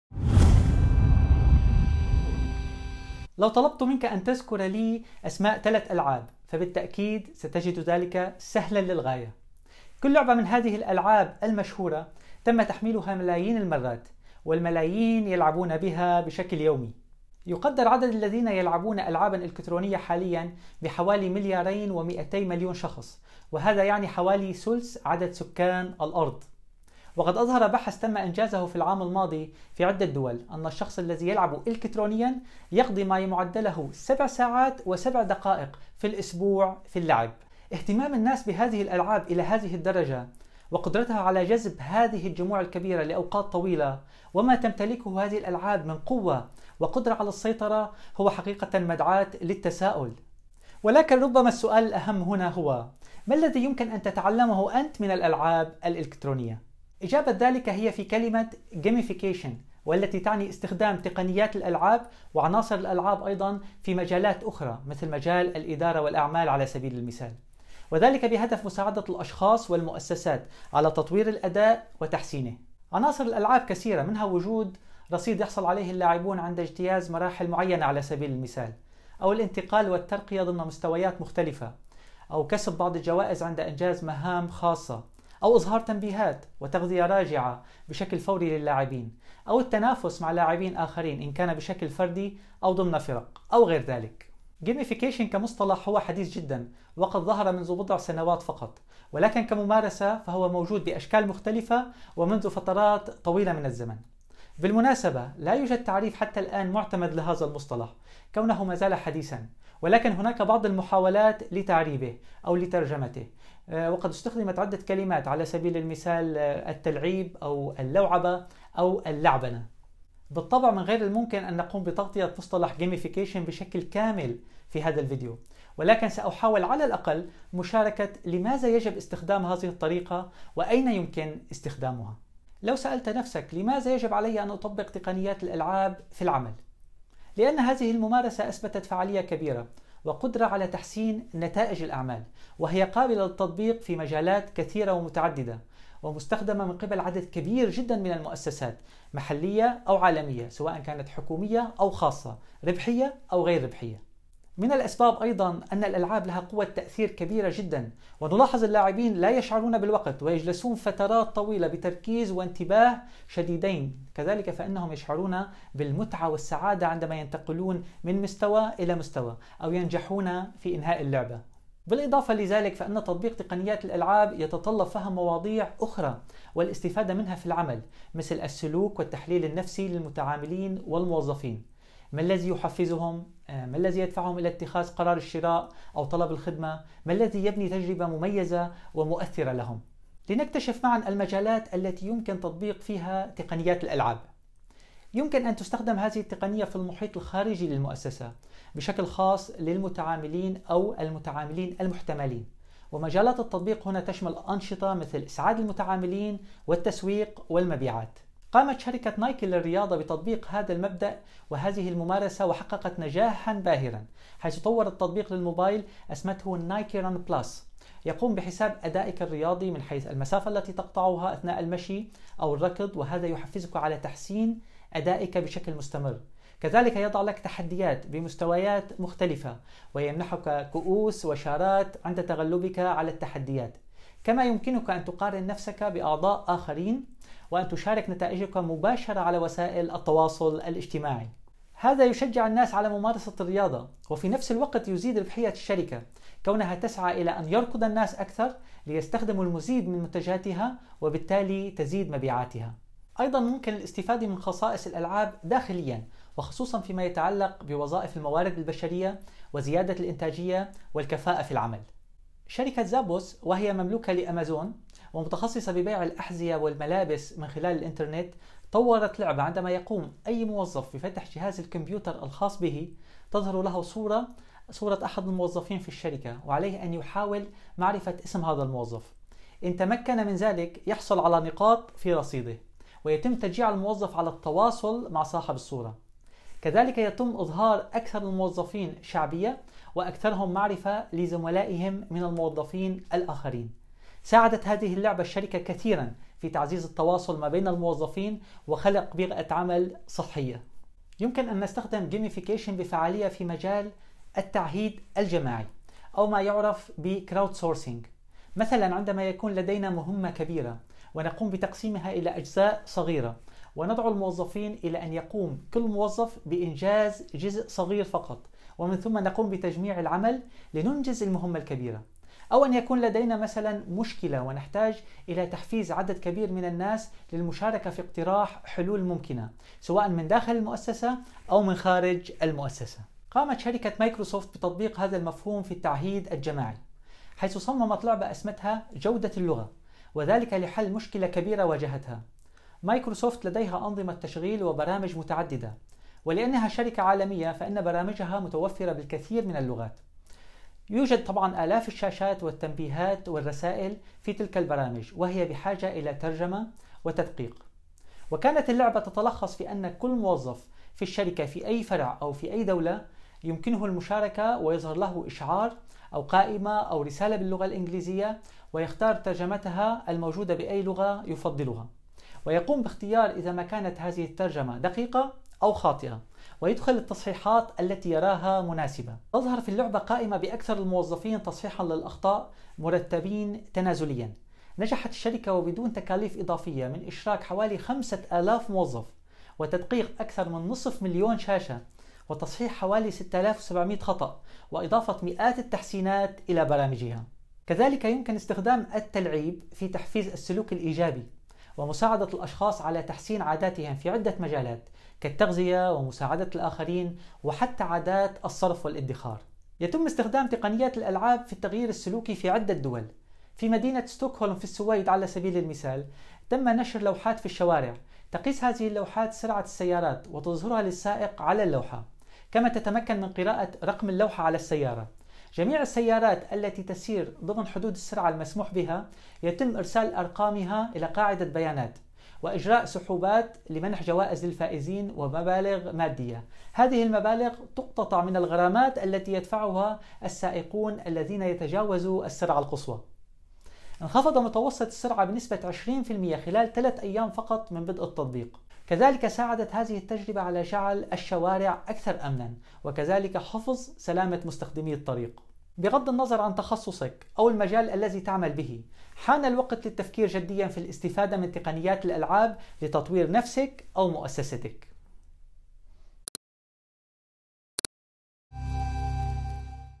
استخدم الألعاب لتطوير نفسك ومؤسستك
التلعيب، اللوعبة، أو اللعبنة تعني شيء واحد وهو استخدام تقنيات وعناصر الألعاب في مجالات آخري مثل مجال الإدارة والأعمال.. وذلك بهدف مساعدة الأشخاص والمؤسسات على تطوير الأداء وتحسينه.
عناصر الألعاب كثيرة منها وجود رصيد يحصل عليه اللاعبون عند اجتياز مراحل معينة، الانتقال والترقية ضمن مستويات مختلفة، كسب بعض الجوائز عند إنجاز مهام خاصة، إظهار تنبيهات وتغذية راجعة بشكل فوري، التنافس مع لاعبين أخرين بشكل فردي أو ضمن فرق، وغير ذلك.
يمكن أن تطبق هذه التقنية في المحيط الخارجي للمؤسسة وبشكل خاص للمتعاملين أو المتعاملين المحتملين ومجالات التطبيق هنا تشمل أنشطة مثل إسعاد المتعاملين والتسويق والمبيعات.
أيضاً ممكن الاستفادة من خصائص الألعاب داخلياً وخصوصاً فيما يتعلق بوظائف الموارد البشرية وزيادة الإنتاجية والكفاءة في العمل.
كذلك يمكن استخدام التلعيب في تحفيز السلوك الإيجابي ومساعدة الأشخاص على تحسين عاداتهم في عدة مجالات كالتغذية ومساعدة الأخرين وحتى عادات الصرف والادخار.
شارك هذا الفيديو مع من يمكن أن يستفيد لتعم الفائدة.